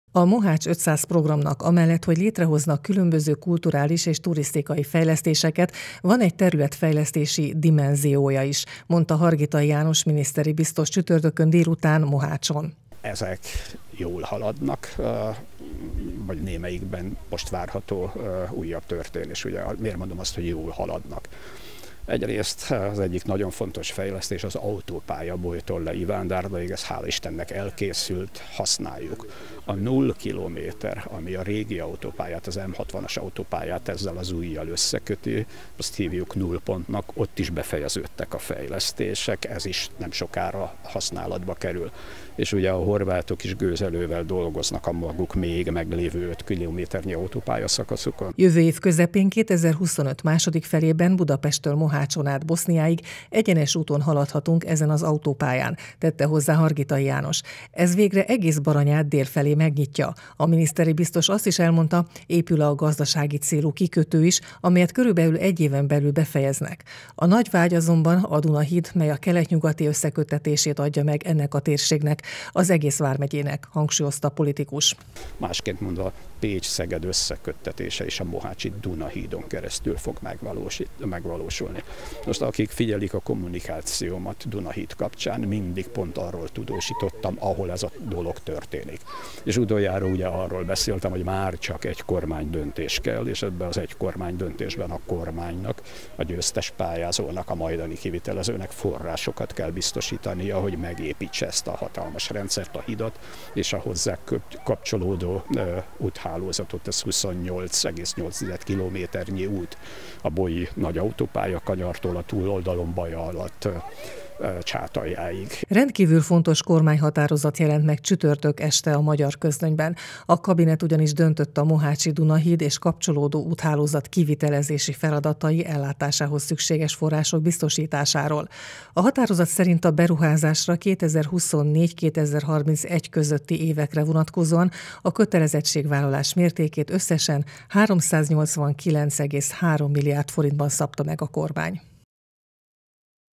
Közel 400 milliárdból épülhet meg a mohácsi Duna-híd - megvan a kormánydöntés
Forrást biztosított a Kormány a mohácsi új Duna-híd beruházására- jelent meg a csütörtöki közlönyben. A beruházásra a 2024–2031 közötti évekre vonatkozóan a kötelezettségvállalás mértékét összesen 389,3 milliárd forintban szabta meg a kormány. A projketről Hargitai János miniszteri biztos, országgyűlési  képviselő beszélt stábunknak.